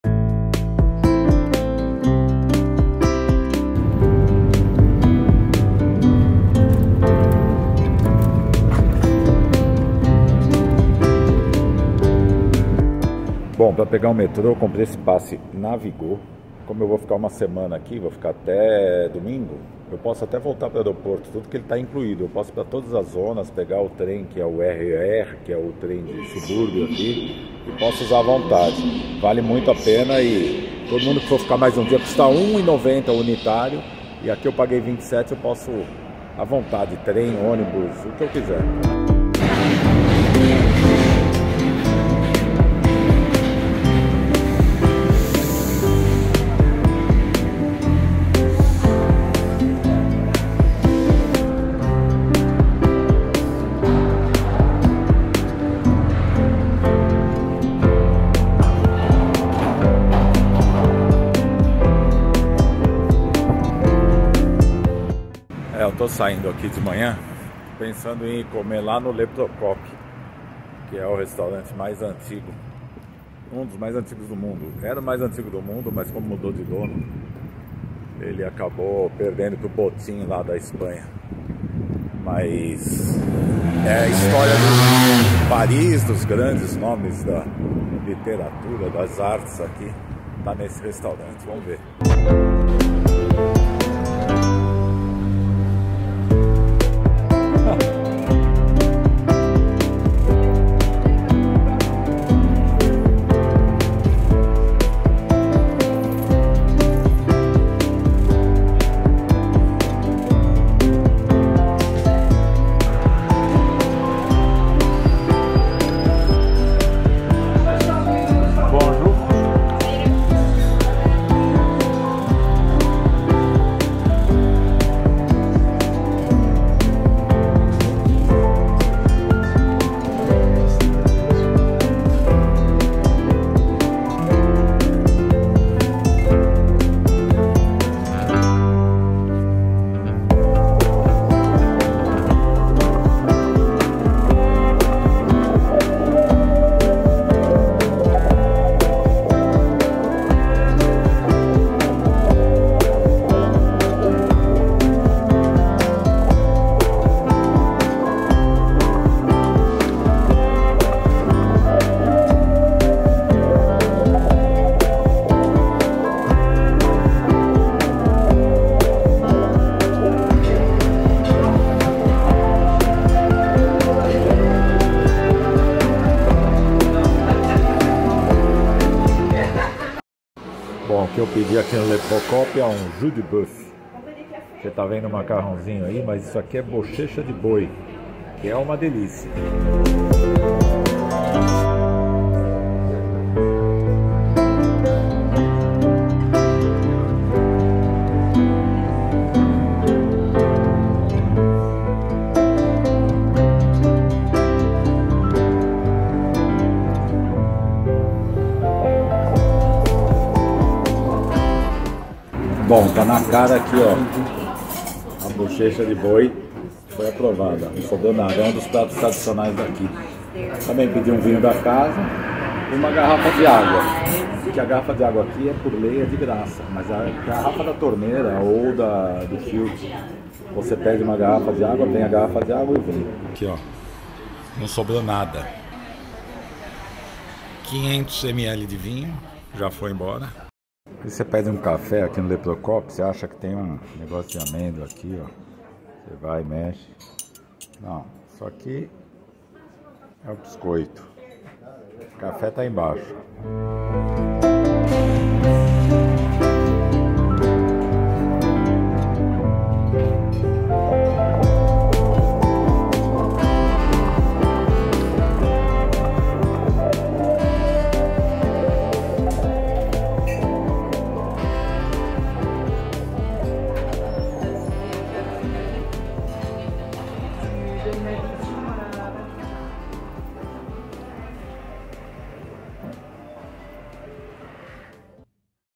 Paris - Onde comer - Restaurante Le Procope - Fundado em 1686 foi o mais antigo do mundo
O Café Procope, na rue de l'Ancienne Comédie, 6.º bairro de Paris, é considerado o café mais antigo de Paris em operação. Foi inaugurado em 1686 pelo chef siciliano Procopio Cutò (também conhecido pelo seu nome italiano de Francesco Procopio dei Coltelli e pelo seu nome francês de François Procope),  e foi um local de encontro da comunidade artística e literária de Paris nos séculos XVIII e XIX. O café original fechou em 1872 e só voltou a reabrir como café na década de 1920. Esses anos fechados fizeram com que o restaurante perdesse o título de mais antigo do mundo pelo Sobrino Botin de Madrid que é de 1725 e nunca fechou as portas.